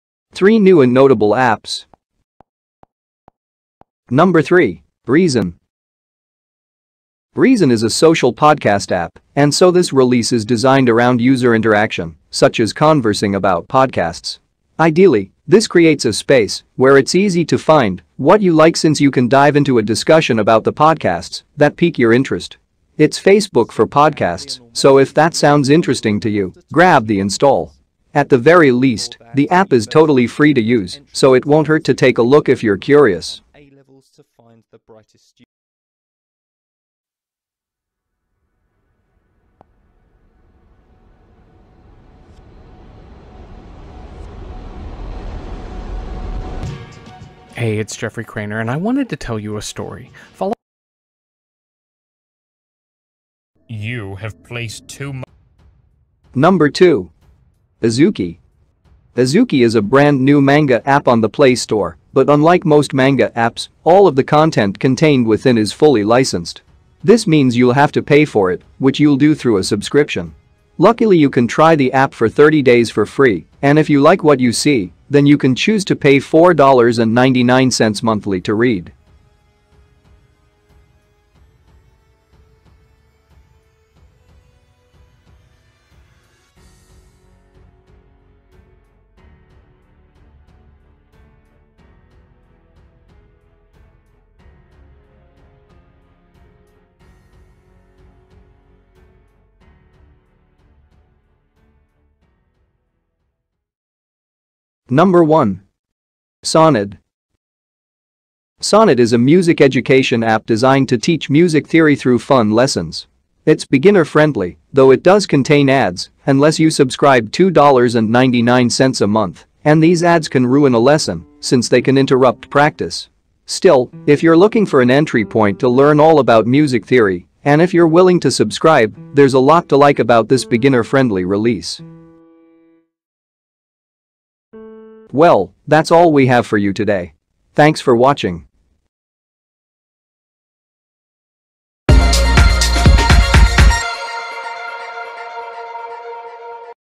3 New and Notable Apps
This video contains 3 New Apps that launched recently which you must try.

3. Reason

2. Azuki

1. Sonid

Timecodes:
0:00 - Reason
0:55 - Azuki
1:44 - Sonid
2:21 - End Credits